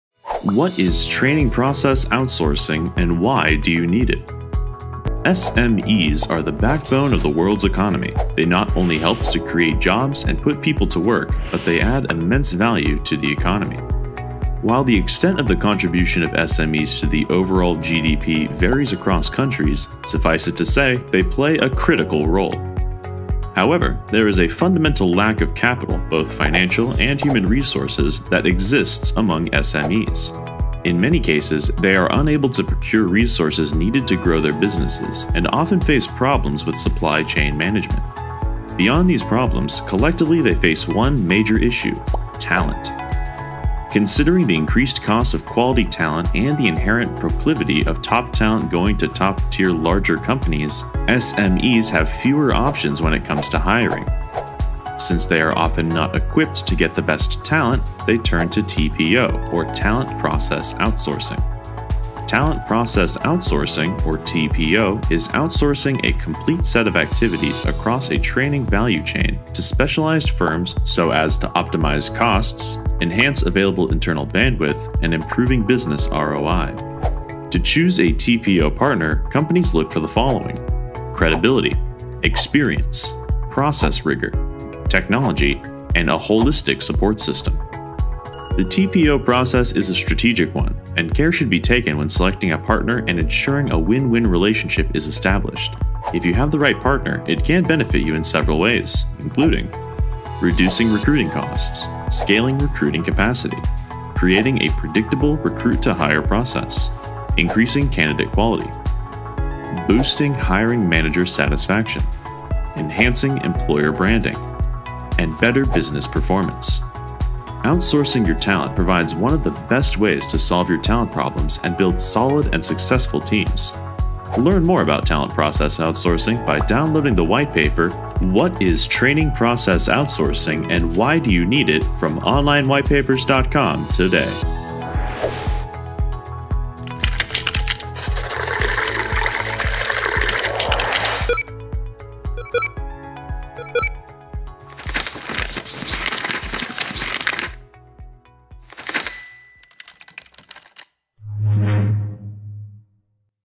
What Is Training Process Outsourcing and Why Do You Need It
SMEs are the backbone of the world’s economy. They not  only help to create  jobs and  put people  to  work  but  they  add  immense  value  to  the economy.  While  the  extent  of the contribution of SMEs to the overall GDP varies across countries, suffice it to say they play a critical role.